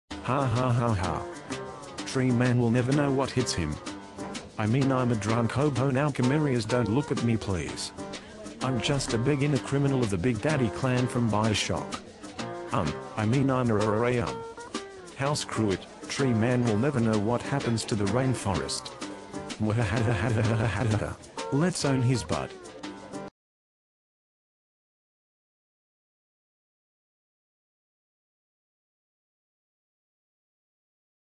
Revenge Of Da Zoo Keeper
Revenge of da evil zoo keeper from the zoo in notta city. Right by Retard village and the ZA EVIL LAVA FRENCH CAKE CASTLe OF EVIL FRENXH PEOPLZ. I mean evil castle for short.